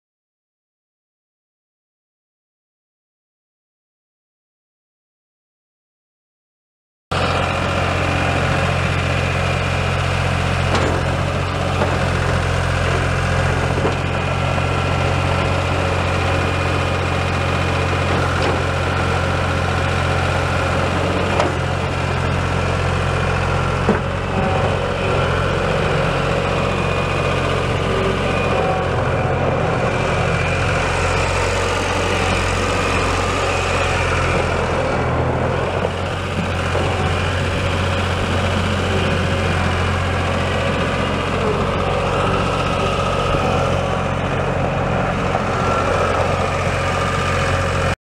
1998 New Holland LX885 skid steer for sale | sold at auction December 18, 2014
SOLD! NoMug Construction Estate Auction

1998 New Holland LX885 skid steer
2,882 hours on meter
New Holland 332T 3.2L three cylinder turbo diesel engine
50 kW
Serial 784982
Hydrostatic transmission
Enclosed cab
Heat
Hand and foot controls
EMS panel
Front glass included
Auxiliary hydraulics
Manual quick coupler
Deestone 12-16.5 NHS tires
7,004 lbs. base operating weight
Manual attachment lever broken
Possible coolant leak on radiator
Serial 115118